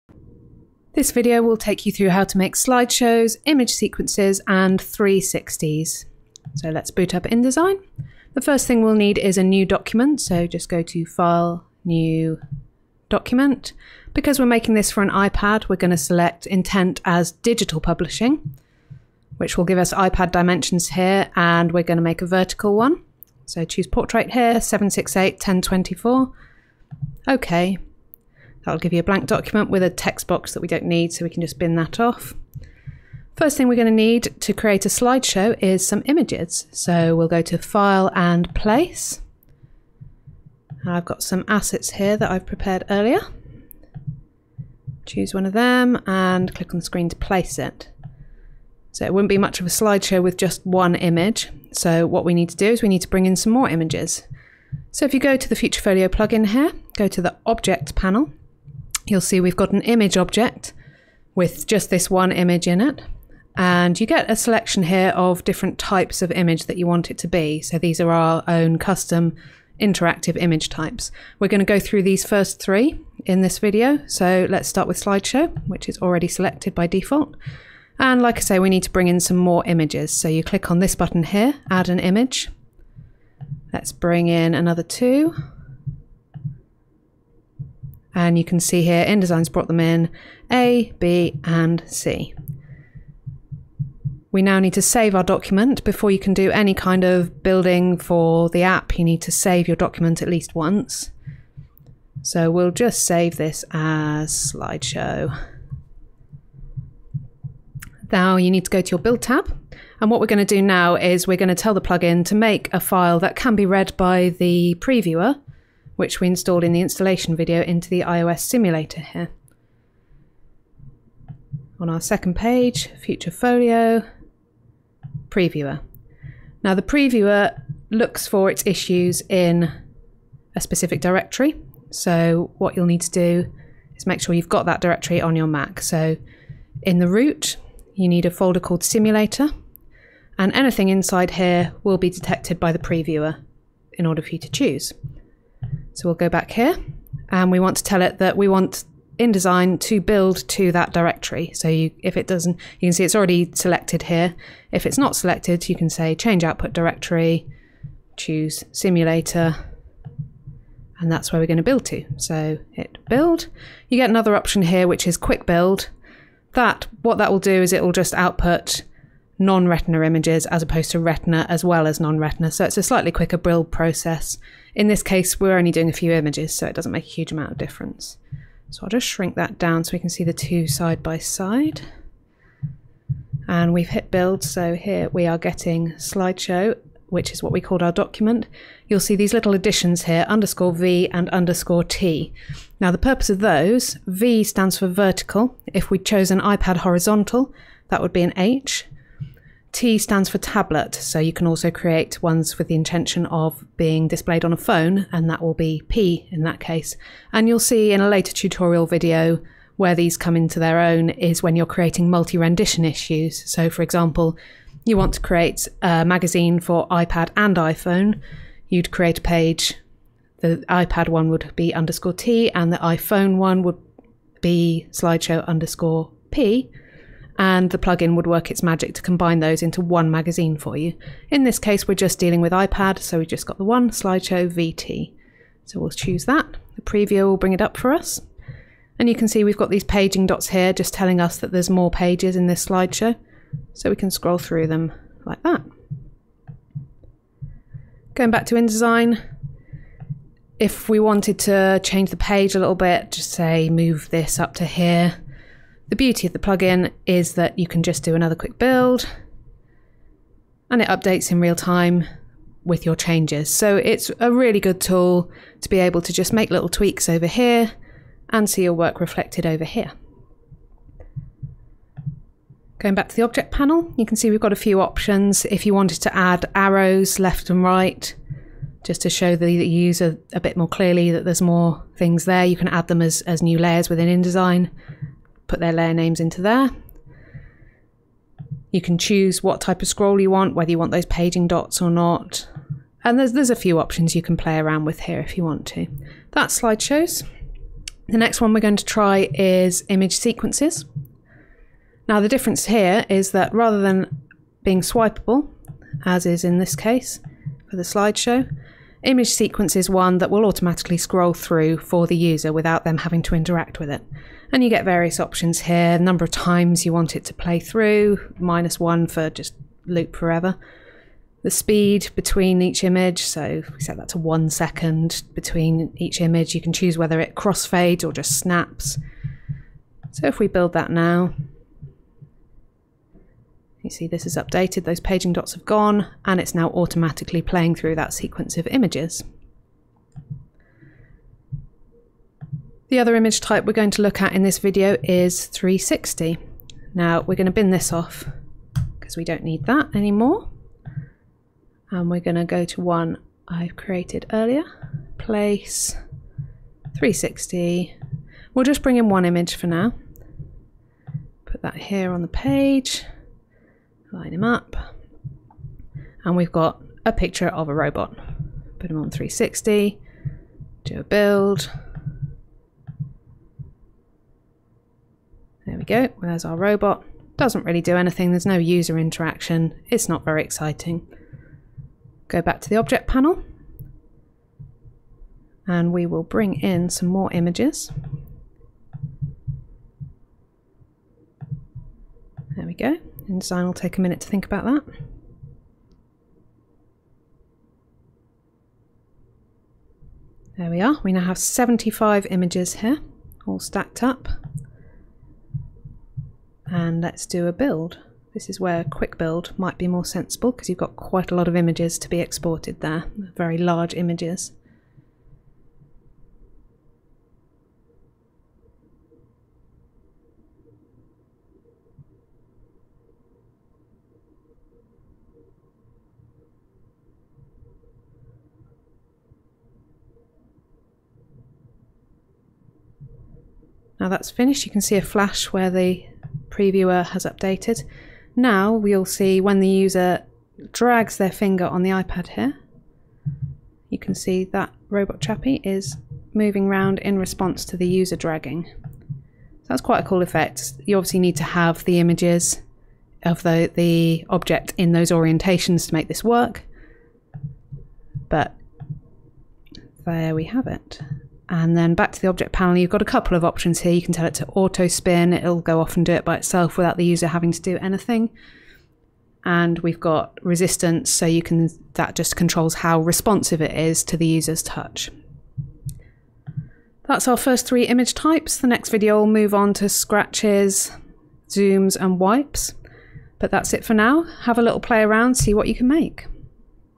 Creating image slideshows, image sequences and 360 rotations
This tutorial video focuses on 3 image types that you can create using FutureFolio:

- Slideshows
- Sequencing
- 360 rotations